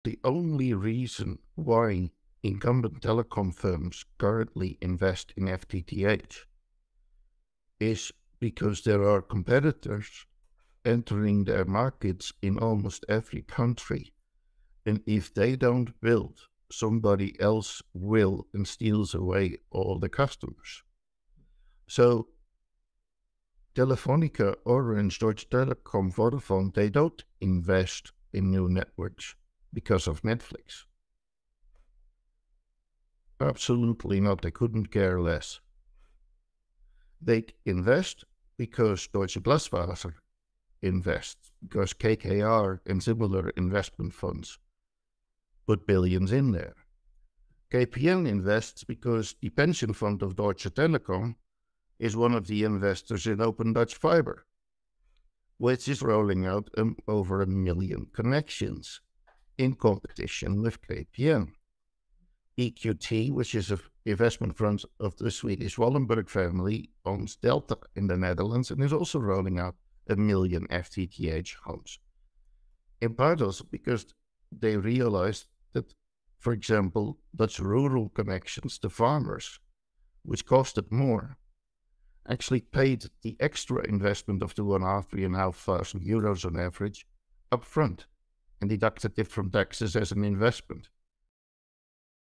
Rudolf van der Berg - Q3 - Competitors Drive Incumbents to Invest
In this podcast Rudolf van der Berg (Stratix) & the internet lab discuss the economic dimension of internet regulation | full episode